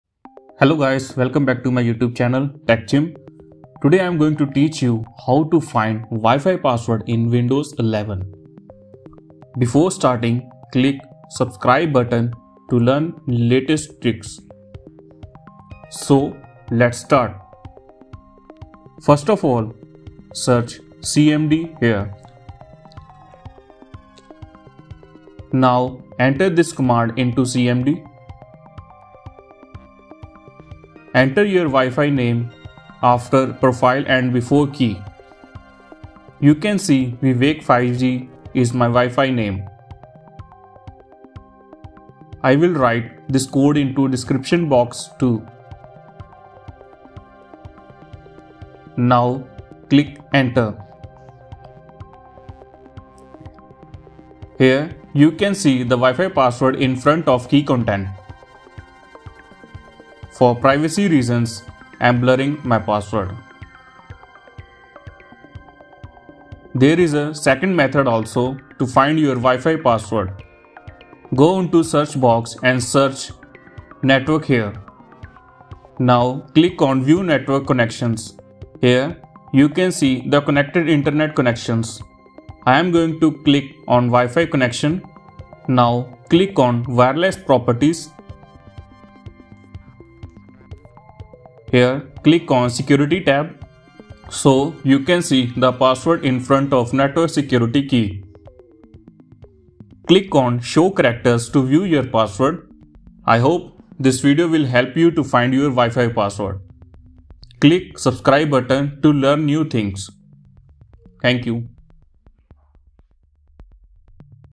How To Find WiFi Password in Windows 2022 ? | Easy Methods
Hello Guys Learn How To Get Wifi Password in Windows 11 | You Can Find Your Wifi Password in Windows 10 Also.

To Find Your Wifi Password Through CMD  Write This Command in Your CMD

NETSH wlan show profile Your Wifi Name key=clear

Don't Forget To Rename Your Wifi Password With Your Connected Wifi Password.